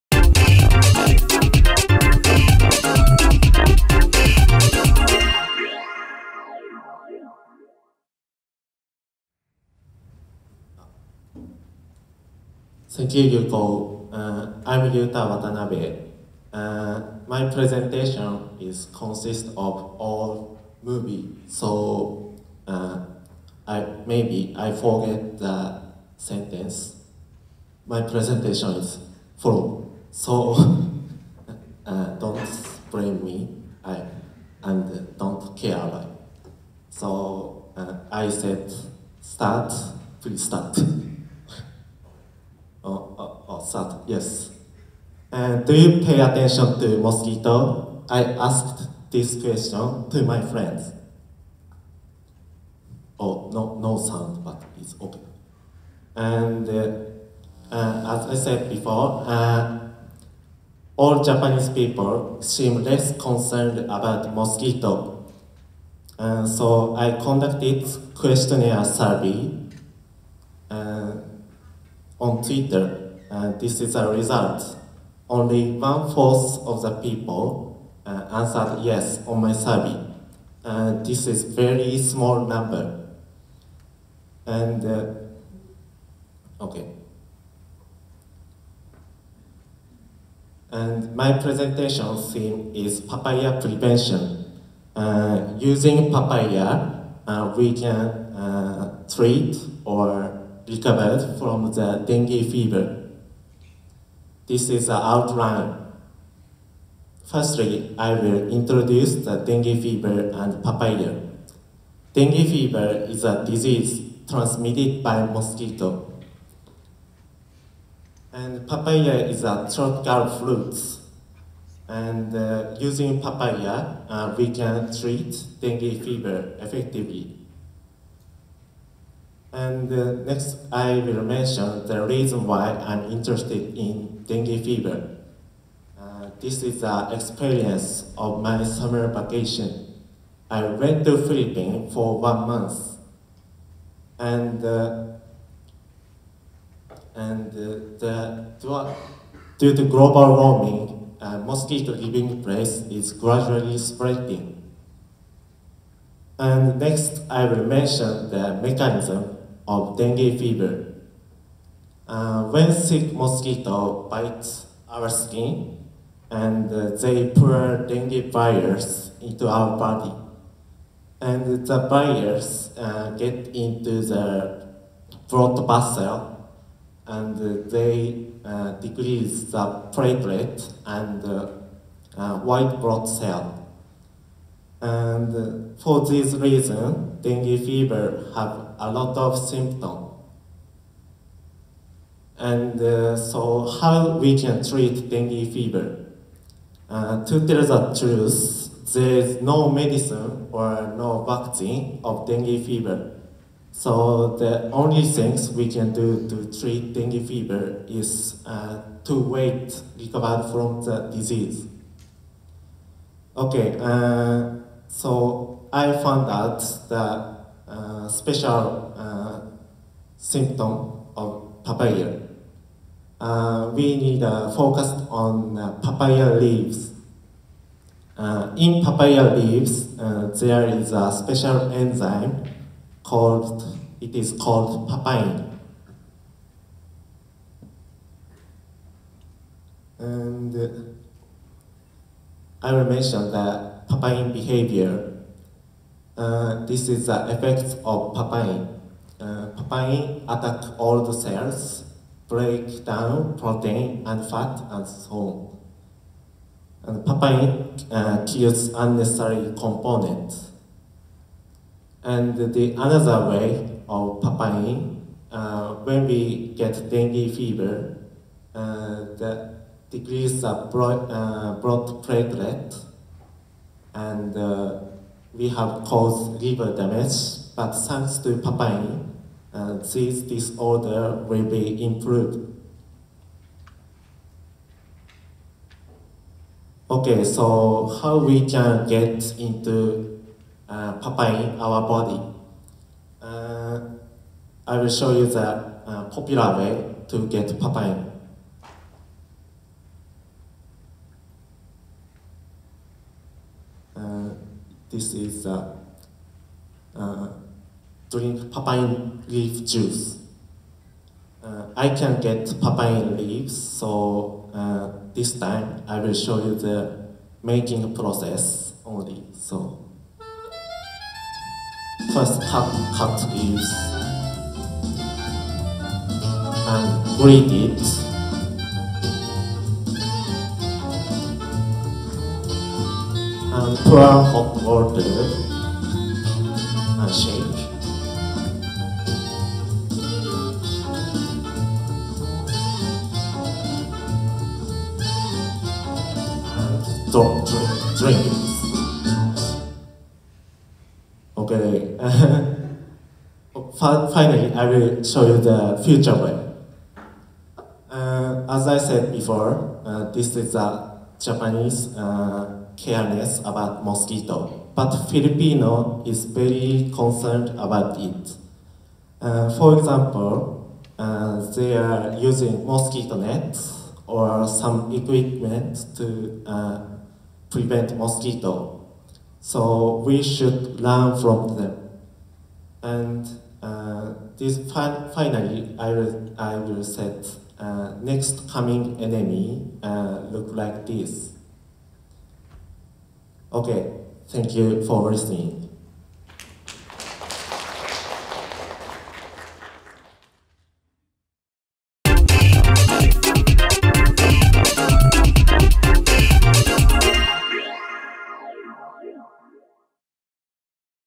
2015 JP2 PEP Talks (Day 1): Yuta WATANABE
立命館大学生命科学部・薬学部で2015年度後期に開講されたプロジェクト発信型英語プログラム授業「Junior Project 2」（JP2）の最終発表の様子です。専門科目の教員と英語教員の助言を受け、両学部の学生たちが英語プレゼンテーションに挑みました。